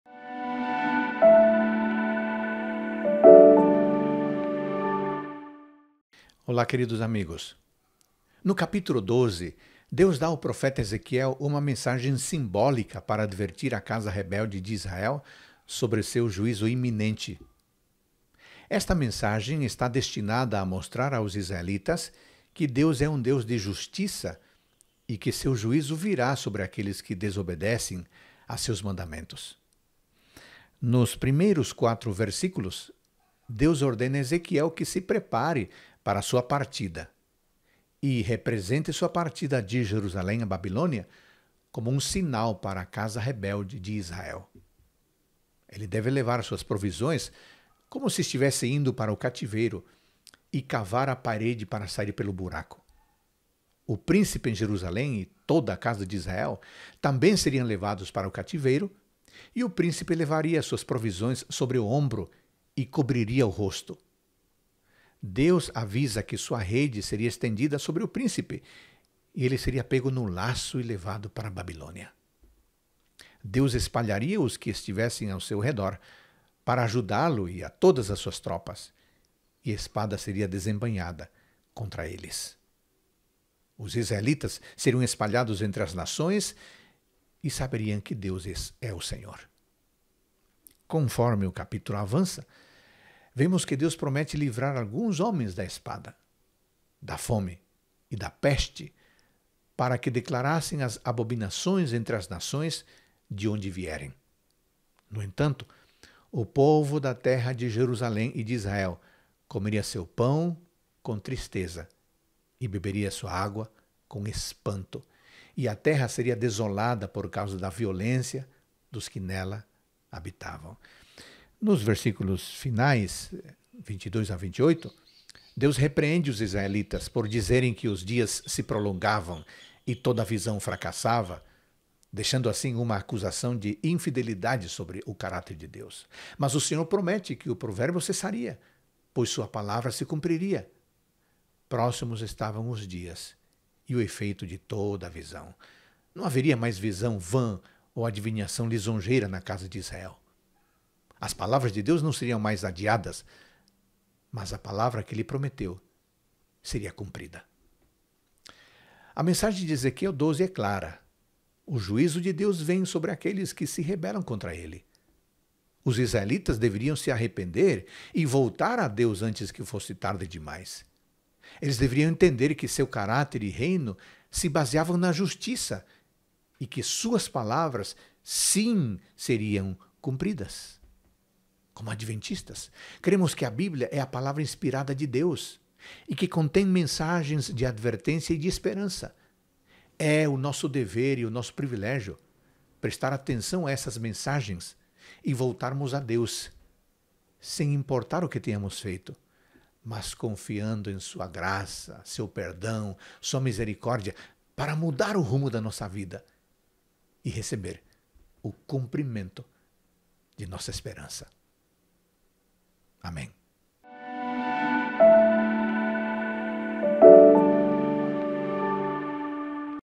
Ezequiel 12 - Reavivados Por Sua Palavra
As dificuldades de hoje servem de preparação para os desafios maiores que enfrentaremos no futuro.